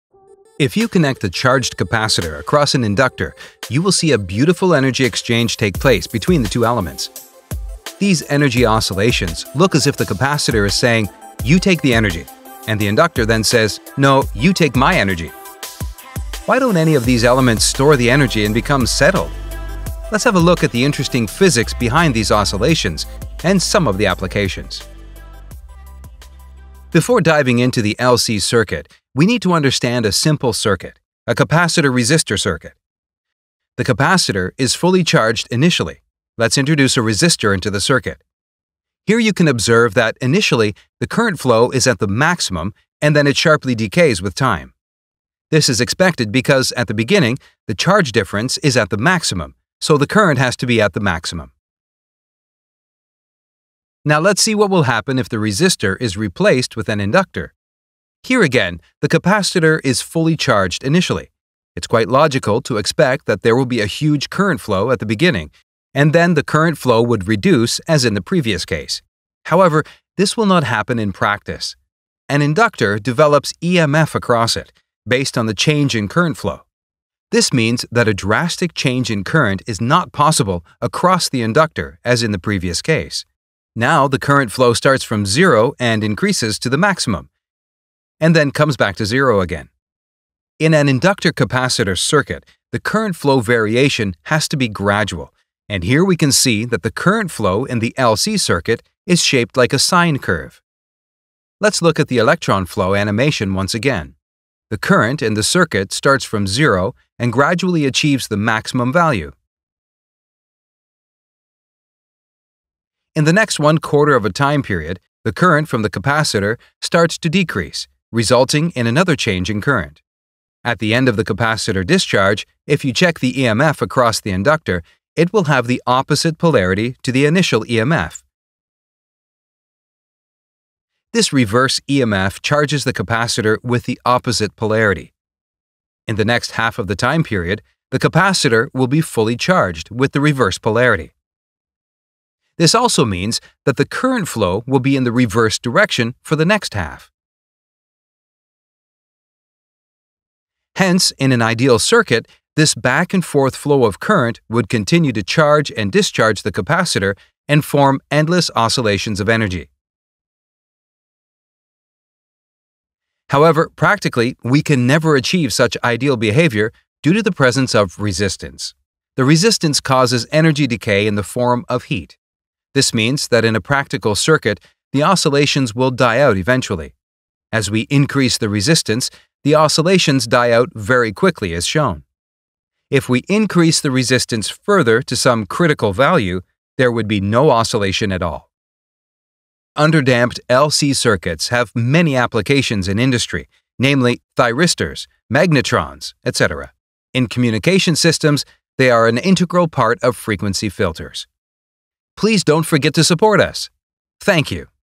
The beauty of LC Oscillations!
If you connect a charged capacitor across an inductor, you will see a beautiful energy exchange take place between the two elements. These energy oscillations look as if the capacitor is saying: ‘you take the energy’ and the inductor then says: ‘no, you take my energy’. Why don’t any of these elements store the energy and become settled? Let’s have a look at the interesting physics behind these oscillations, and some of the applications.